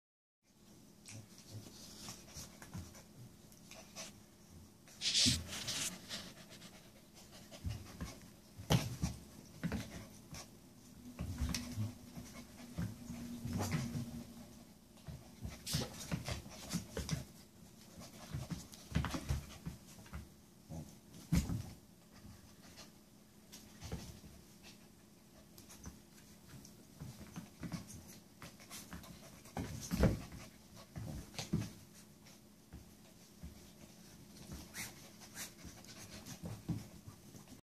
Mouse and Jeannie play big mouth
Puppy Mouse and her Granny Jeannie play "Big Mouth" on the couch.  Sorry Jean, but that couch won't be a safe haven for long.